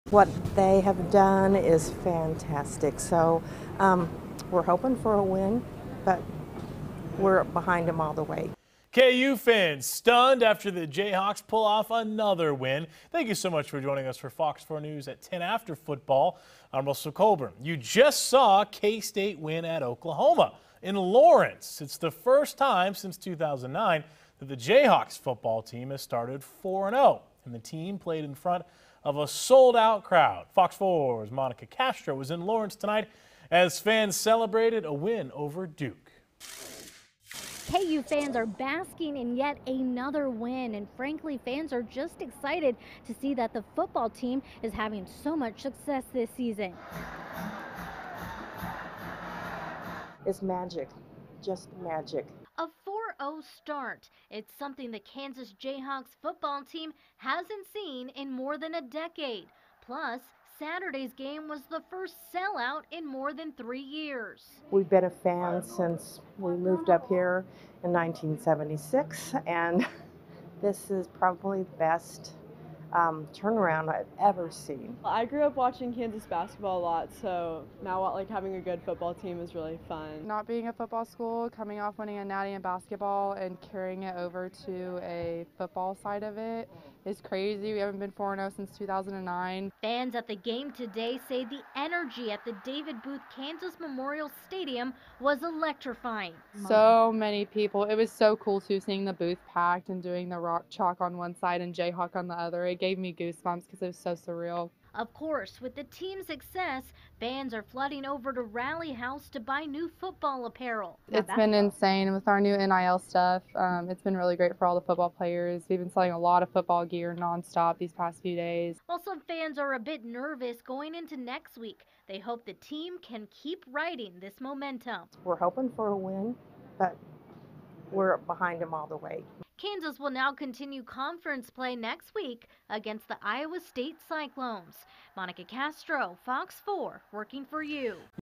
KU fans excited after 4-0 start to the season
Fans are excited to see the Kansas football team having so much success this season.